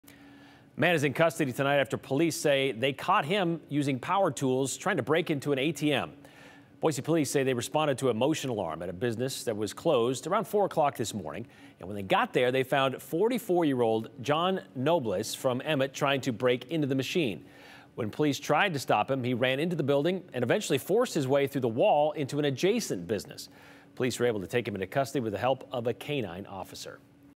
Boise Police stop man using tools on change machines, ATM
An Emmett man is facing two felony charges and one misdemeanor after Boise Police officers caught him breaking into change machines and an ATM Thursday morning.